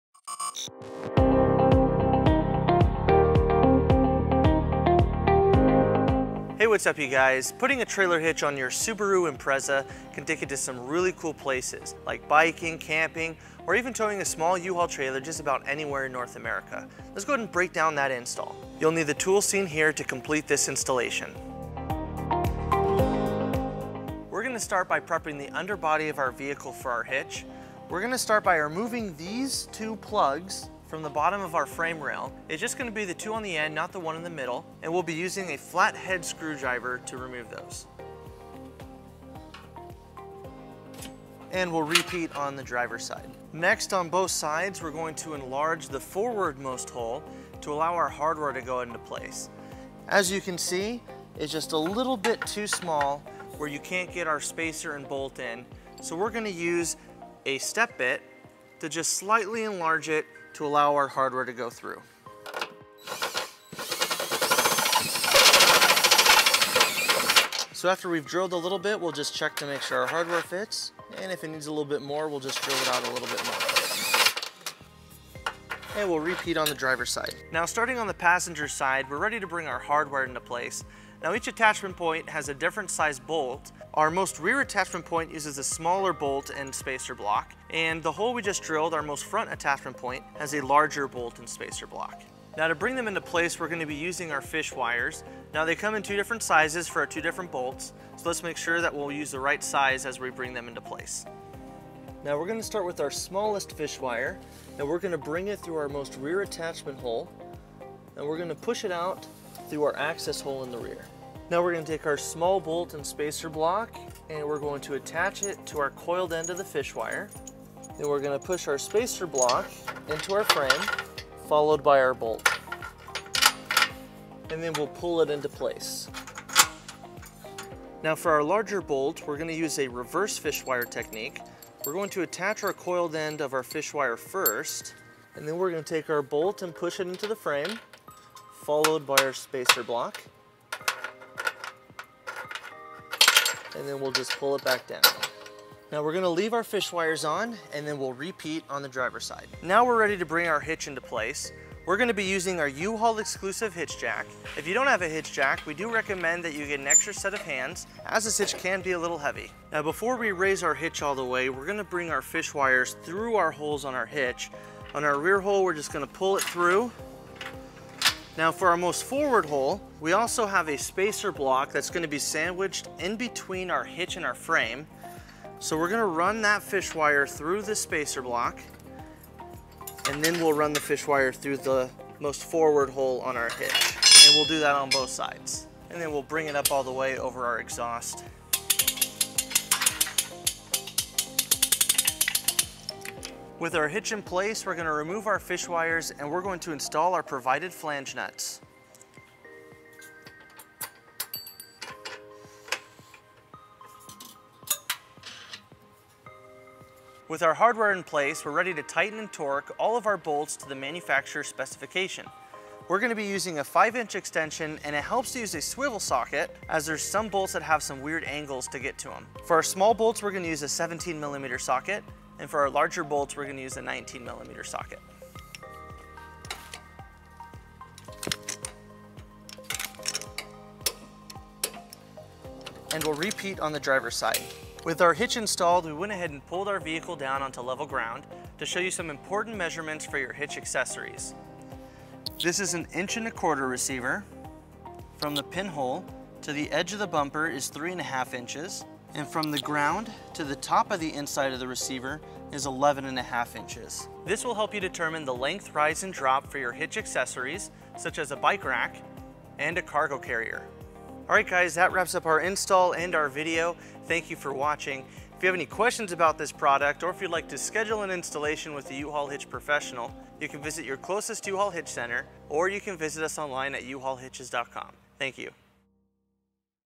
2018 Subaru Impreza Hatchback Trailer Hitch Installation
This video covers the trailer hitch installation for Curt 11465 on a 2018 Subaru Impreza. Learn how to install your receiver on your 2018 Subaru Impreza from a U-Haul installation professional. With a few tools and our step-by-step instructions, you can complete the installation of Curt 11465.

To purchase this trailer hitch and other towing accessories for your 2018 Subaru Impreza,

0:00 Intro
0:19 Tool list
0:24 Install carriage bolts
2:29 Raise hitch into place
3:27 Torque bolts
3:55 Hitch details and measurements
4:30 Outro